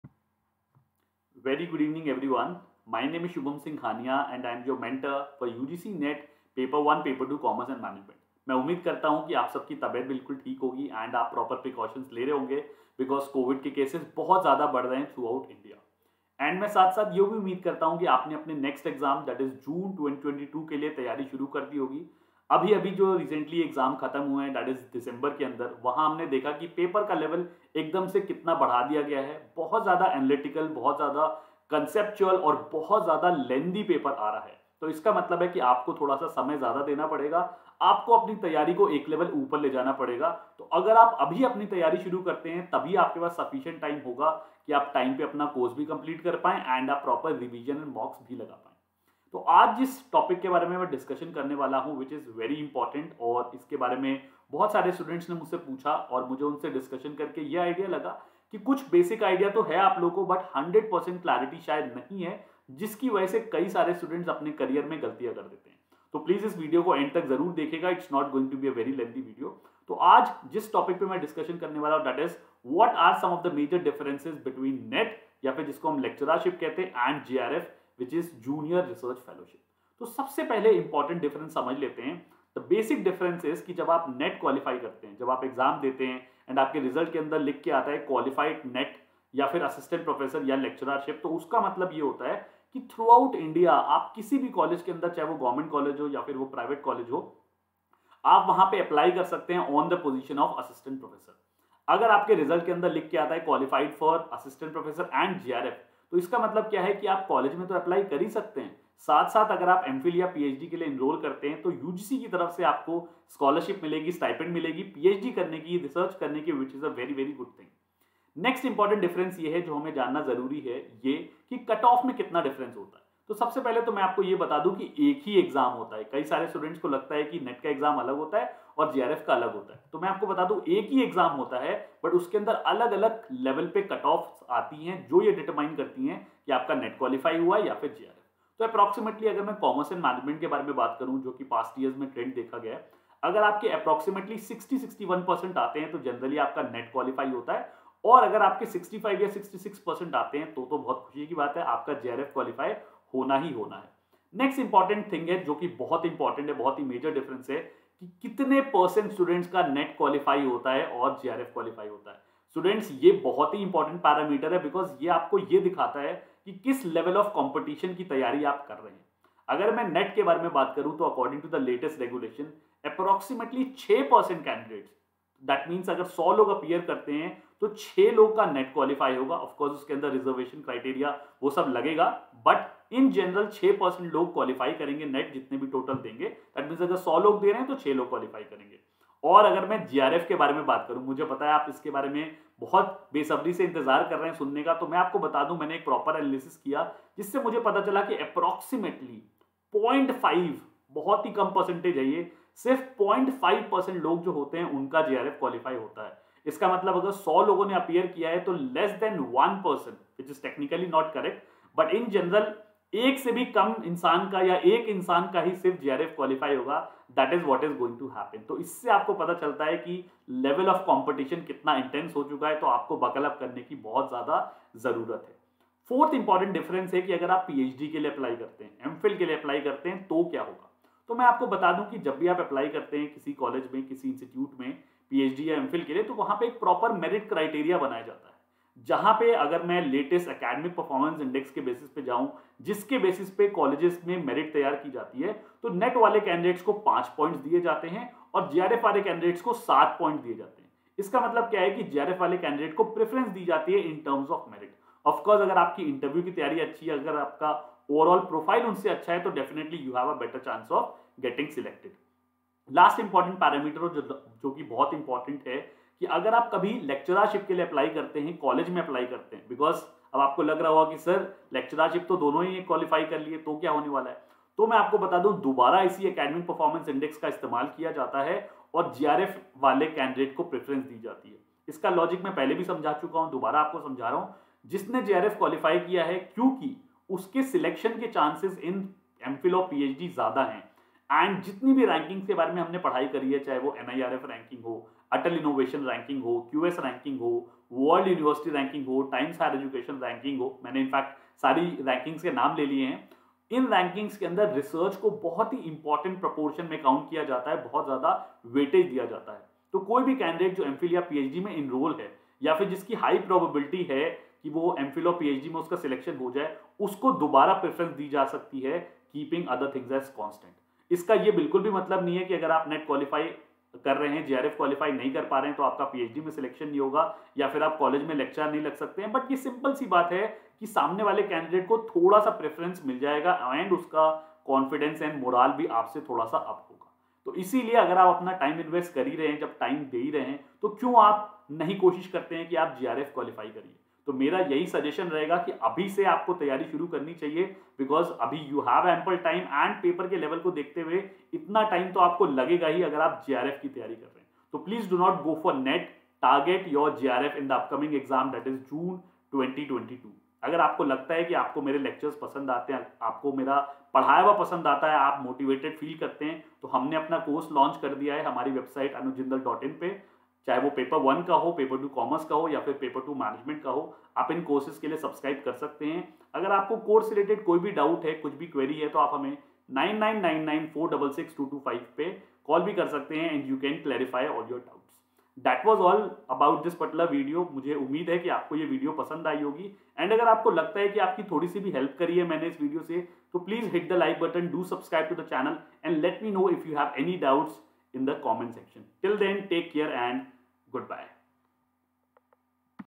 LS/NET VS JRF | What is the Difference ? - Shubham Sir
Hello Students,

I see a lot of Students being confused between NET and JRF, so in this video based on 5 parameters we shall discuss the major differences and what are the advantage of JRF over NET. Do watch the video for better clarity!